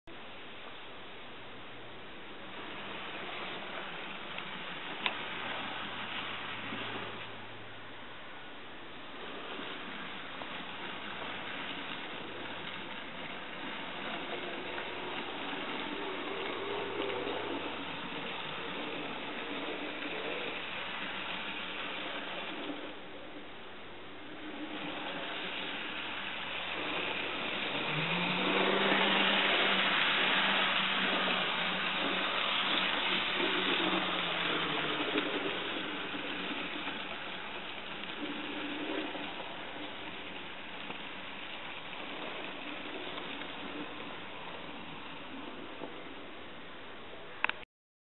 Graham Farish Castle Class DCC Conversion
This is a Graham Farish Castle Class DCC Conversion. Loco is converted using a digitrax dz125 decoder located in the tender and a digi-hat. Excellent running from a Poole Chassis